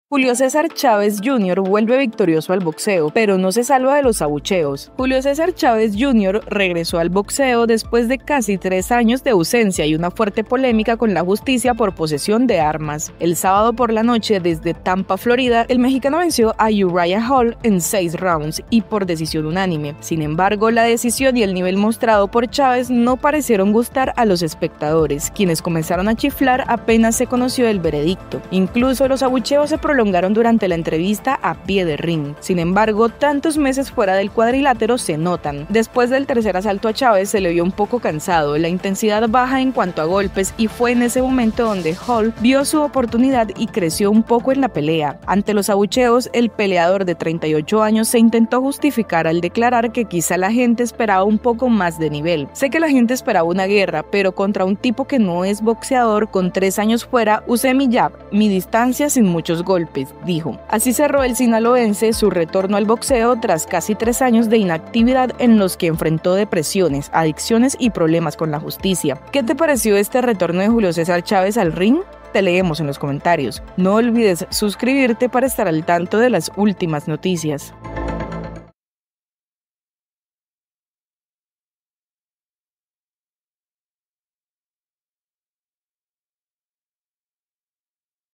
Julio César Chávez Jr. vuelve al ring en medio de abucheos y una victoria | La Opinión
Julio César Chávez Jr. volvió al ring tras casi tres años de ausencia y una polémica por posesión de armas, venciendo a Uriah Hall en Tampa, Florida. Aunque la victoria no convenció a los espectadores, quienes abuchearon el veredicto y su entrevista posterior, Chávez justificó su actuación mencionando los meses fuera del cuadrilátero y el uso de su jab y distancia en lugar de una pelea intensa.

La noticia está en todas partes, y tú eres parte de ella. 🌐 Visita